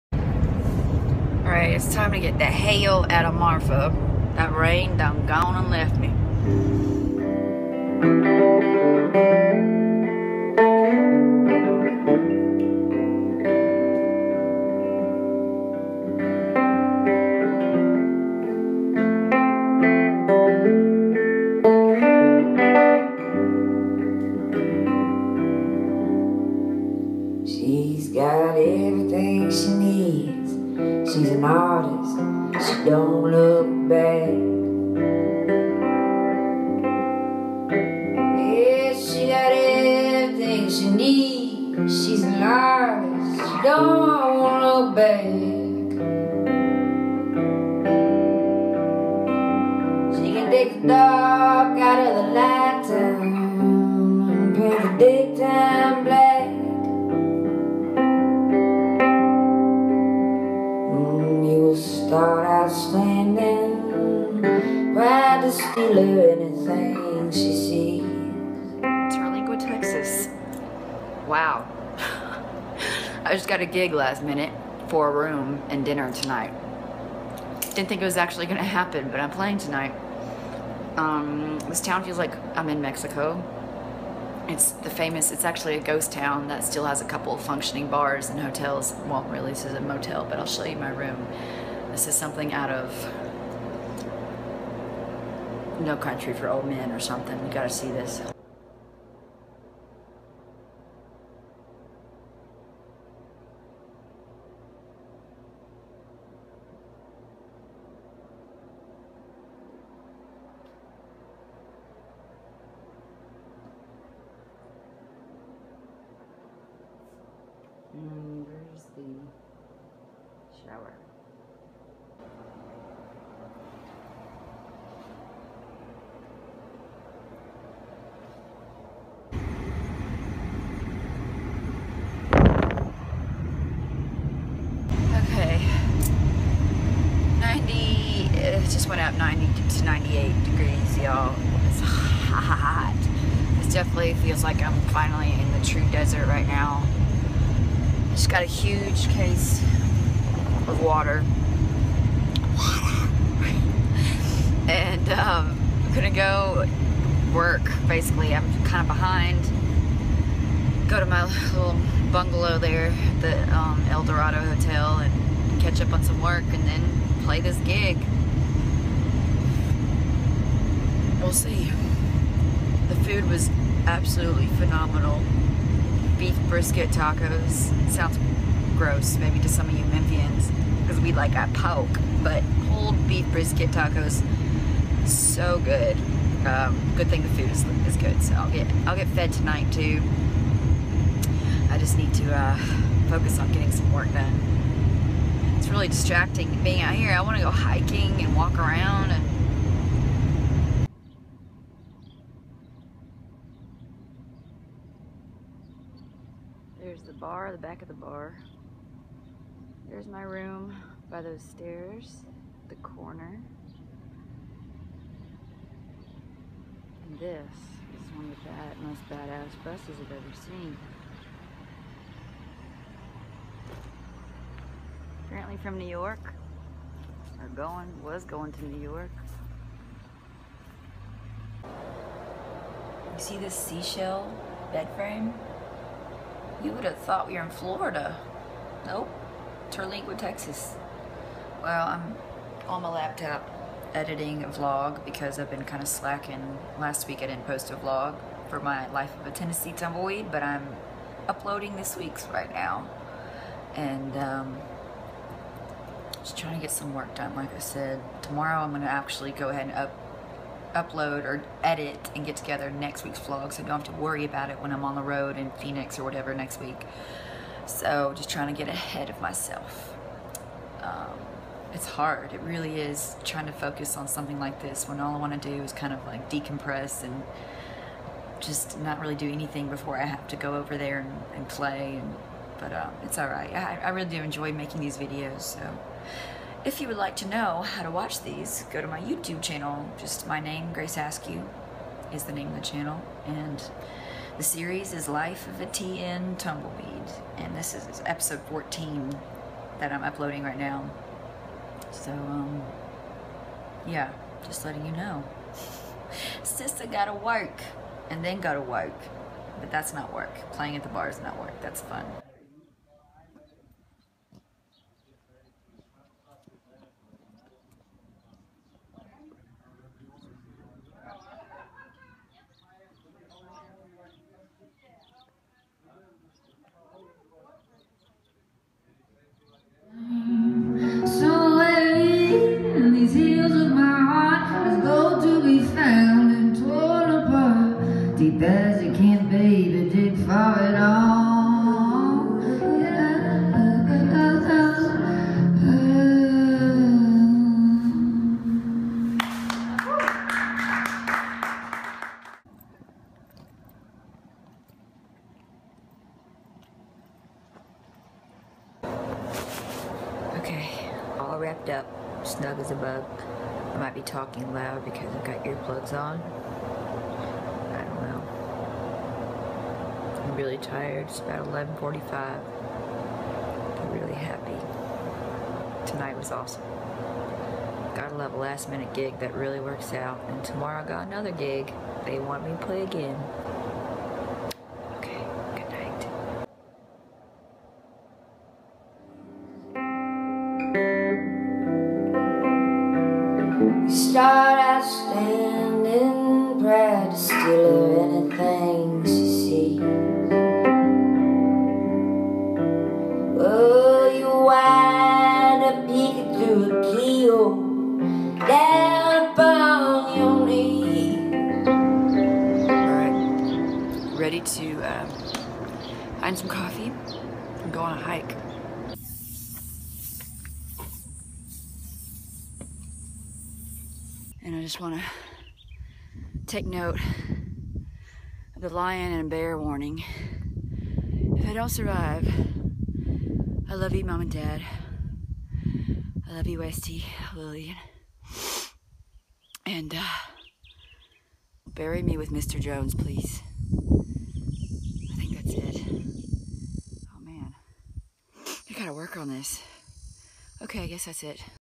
EP 33: GHOST TOWN GIG (TERLINGUA, TX)
"The Life of a TN Tumbleweed" Vlog Series creator/Musician, Grace Askew, takes you along for the 2015 summer tour.

Episode 33: We are back down in deep Southwest Texas for Grace's personal favorite part of this tour..2 nights in Terlingua, a ghost town in the desert land of Big Bend National Park.  A last-minute gig at the famed El Dorado hotel was kindly booked, thanks to Deanna, with all of the wonderful town locals welcoming the tumbleweed with open arms for one of the most unforgettable gigs to date.